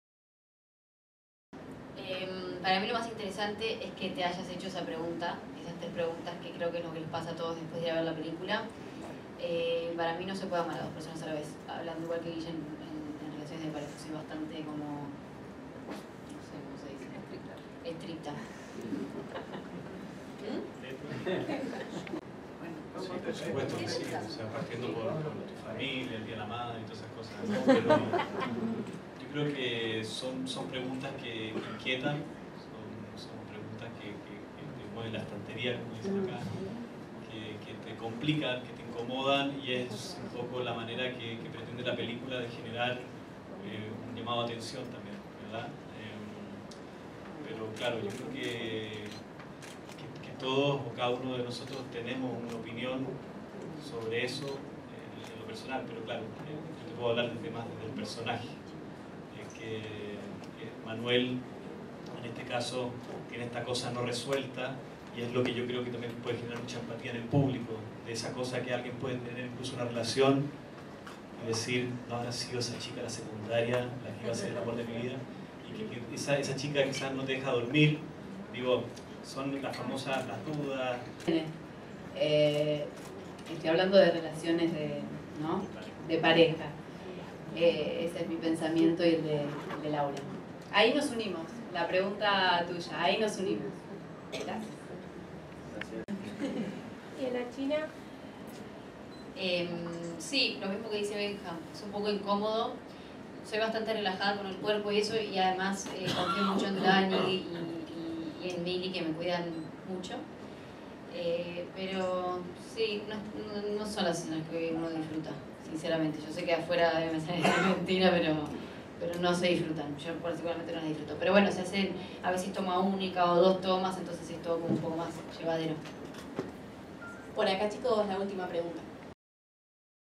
Las preguntas más incómodas en la conferencia de 'El hilo rojo'
Benjamín Vicuña y la China Suárez le ponen el pecho a interrogantes sobre la intimidad y las parejas múltiples.